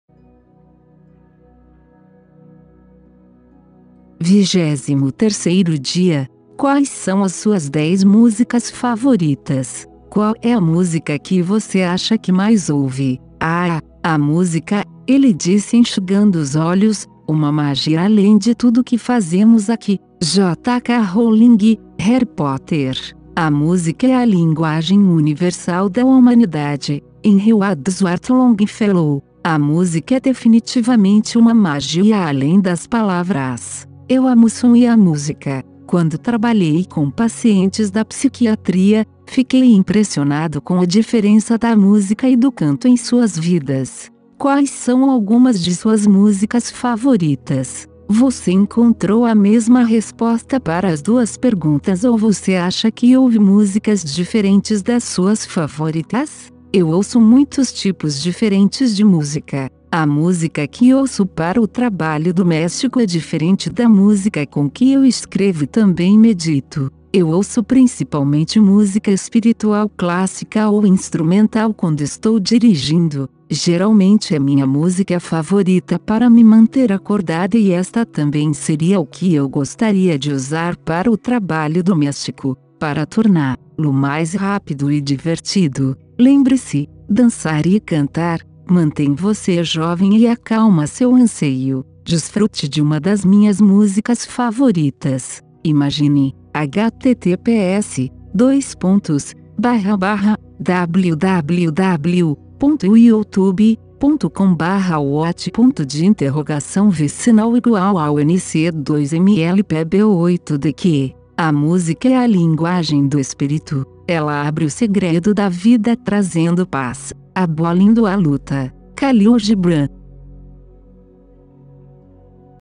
SHARON TAPHORN / ORIENTAÇÃO DOS ANJOS - "UMA JORNADA DE 30 DIAS DE AUTODESCOBERTA" - 23.04.2018
Esta é uma narração da mensagem de   SHARON TAPHORN / ORIENTAÇÃO DOS ANJOS, postada em texto no blog Sementes das Estrelas.